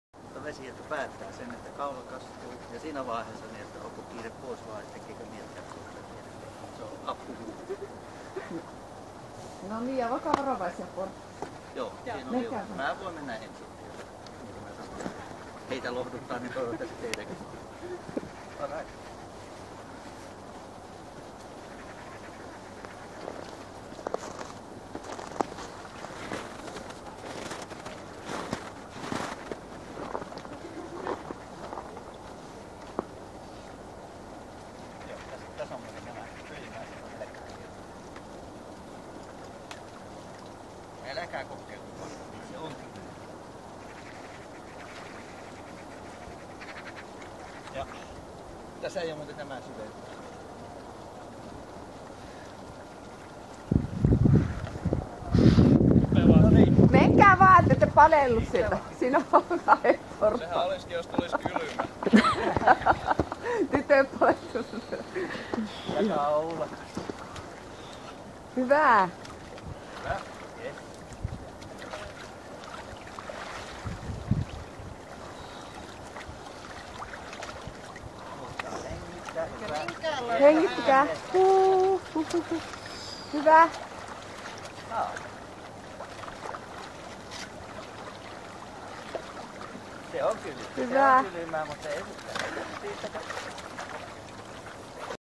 video uinnista
Oulun Luonnontieteilijät ry:n porukkaa kokeilemassa avantouintia Tuiran uimarannalla 4.3.2017 pakkasta -6 astetta ja veden "lämpö" +2 astetta